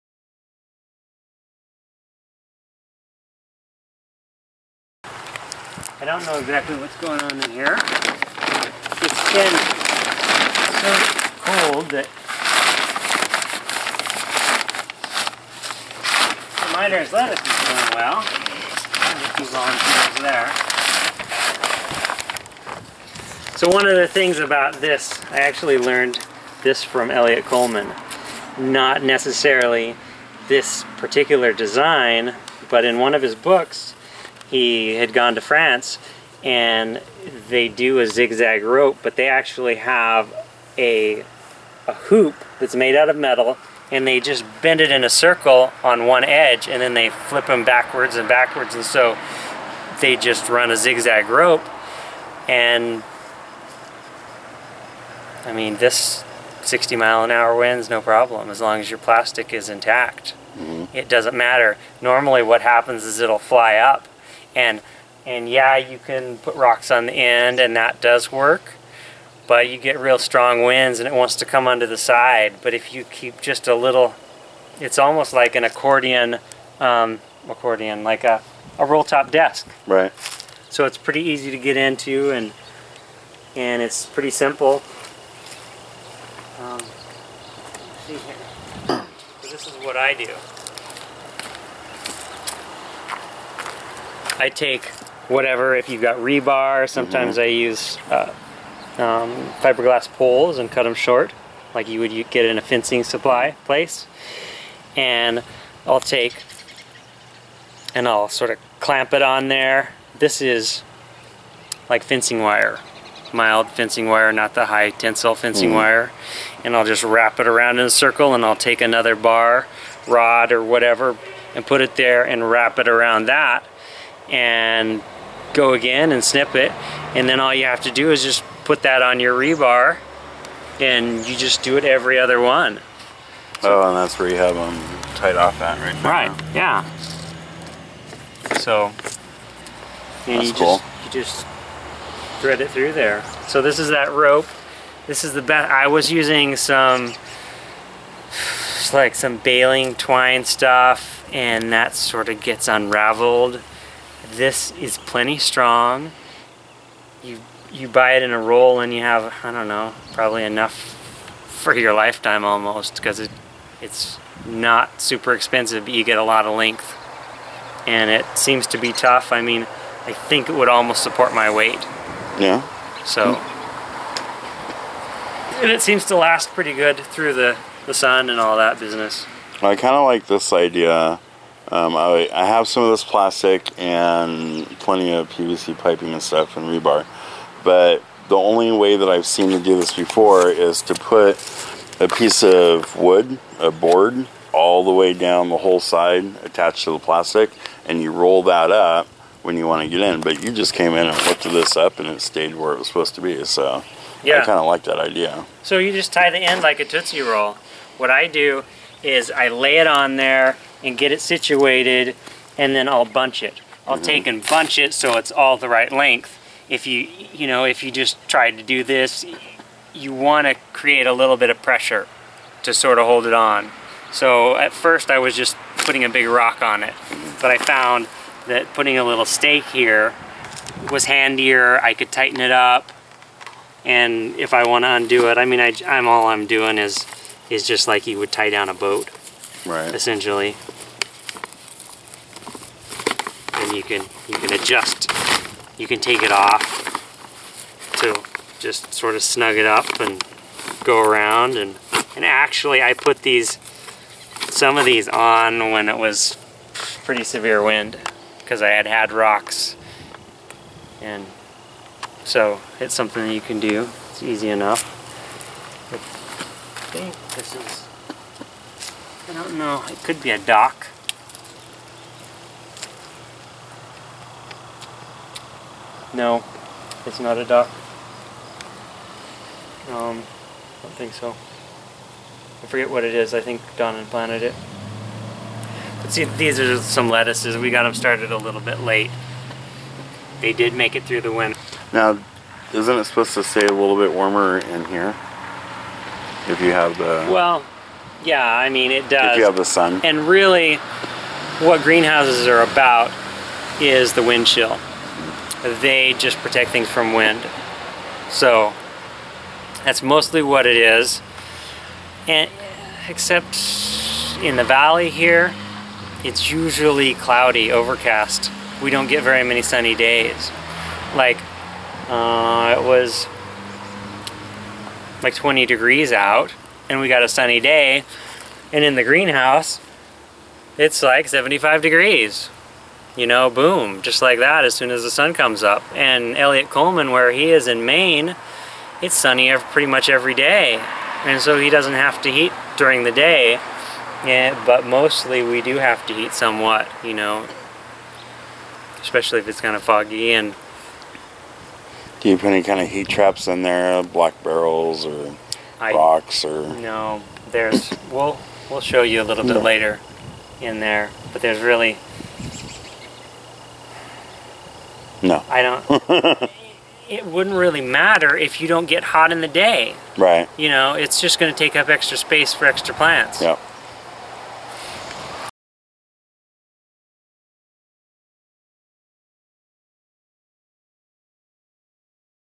Matthew's Hoop House - Subscriber Garden Tour - L2Survive with Thatnub
Mathew shows us his hoop house and explains how he designed the roll top cover.